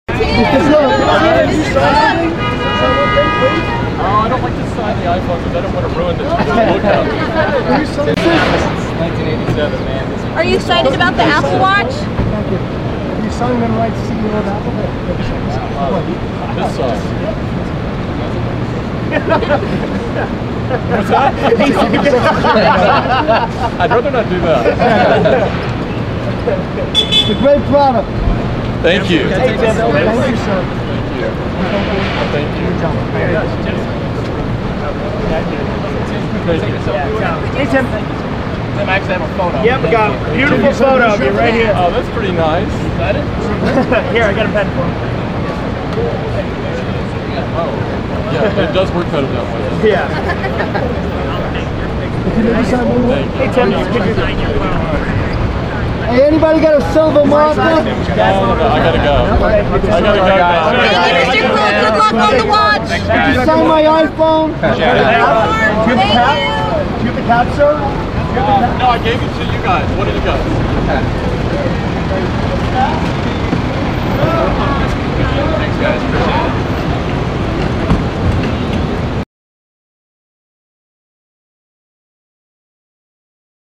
Tim Cook (Apple CEO) after Colbert Show
Tim Cook greets fans after appearing on "The Late Show with Stephen Colbert."